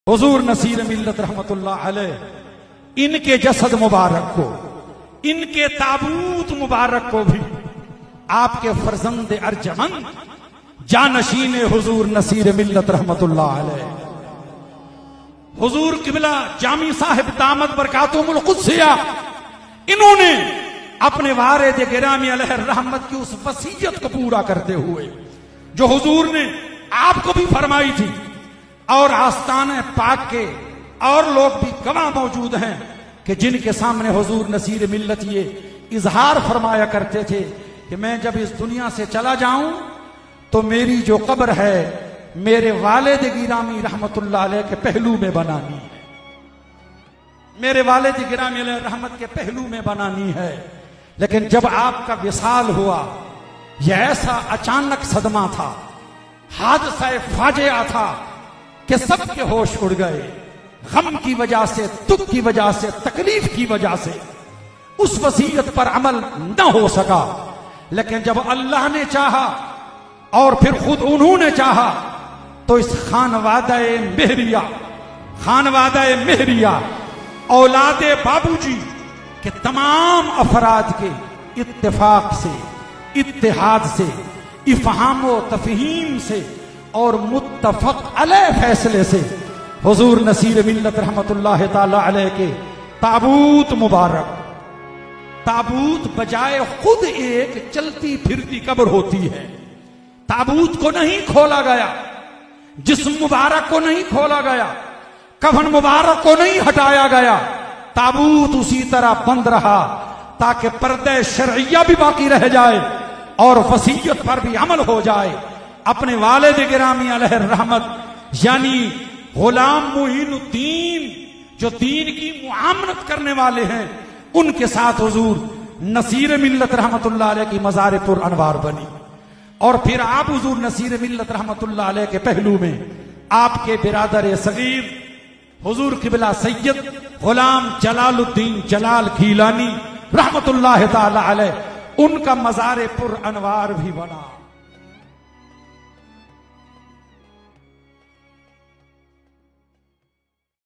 Pir Syed Naseer Ud Din Naseer Shah ki Qabar kushai || Urs E Mubarak Naseer E Millat
پیر صاحب کے عرس کے موقع پہ حضرت قبلہ نصیر ملت پیر سید نصیر الدین نصیر الگیلانی رحمۃ اللّٰہ علیہ کی خواہش کے مطابق آپ کا اور آپ کے بھائی کا مزار ، آپ کے والد گرامی کے پہلو میں سپرد خاک کر دیا گیا ہے۔ سُنئے سب کچھ کیسے عمل میں آیا ہے۔